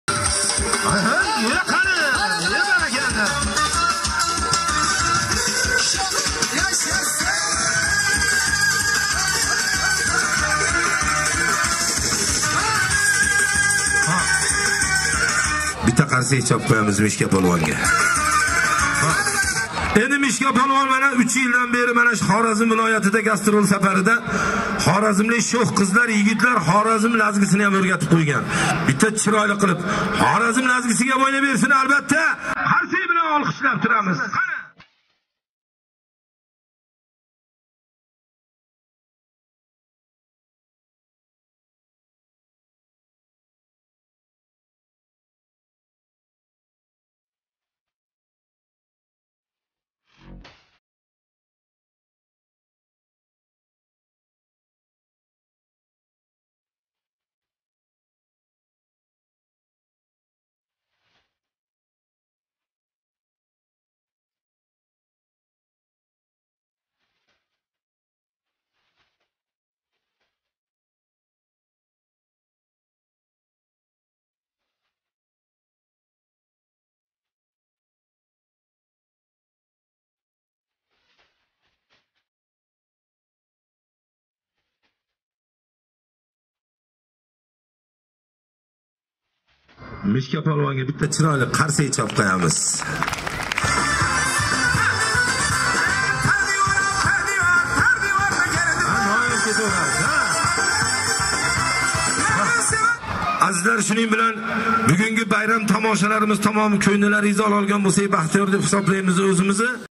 MISHKA POLVON LAZGIGA OʻYNADI
Qo'shko'pir tumanida "Madinabonu" guruhi tomonidan tashkil etilgan dorbozlik tomoshalaridan lavha!